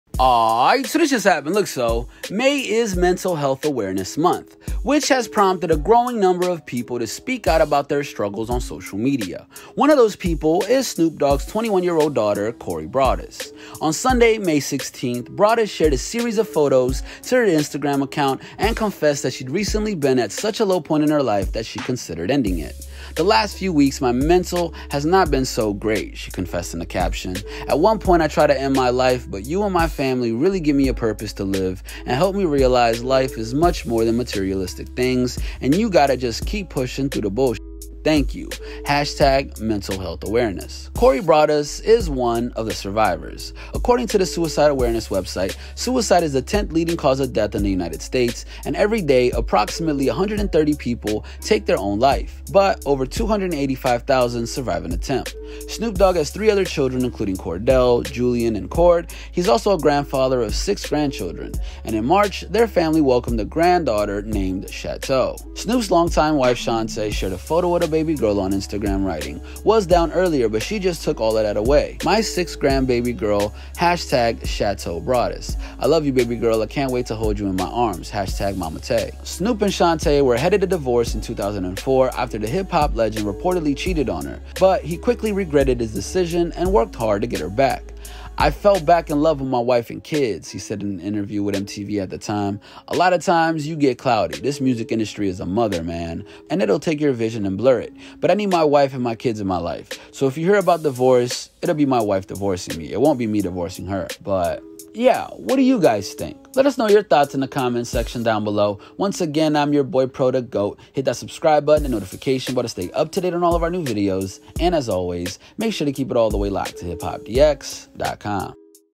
Snoop Dogg's Daughter Opens Up About Recent Suicide Attempt
Script: Kyle Eustice
Voiceover: Pro